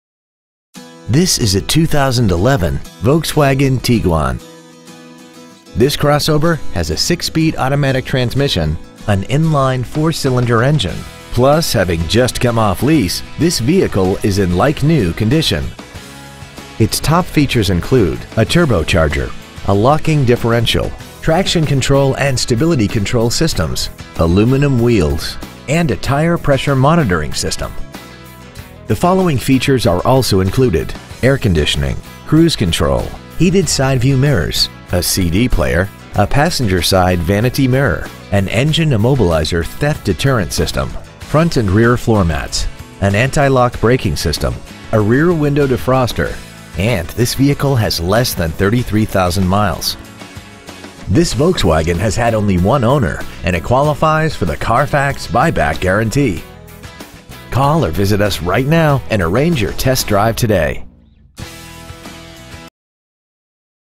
2011 Volkswagen Tiguan Miami FL
Year : 2011
 Make : Volkswagen
 Model : Tiguan
 Engine : Turbocharged Gas I4 2.0L/121
 Trans . : Automatic
 Exterior : Alpine Gray Metallic
 Miles : 32,970
 Interior : Charcoal
 Stock : 45375

 2011 Volkswagen Tiguan Lake Worth FL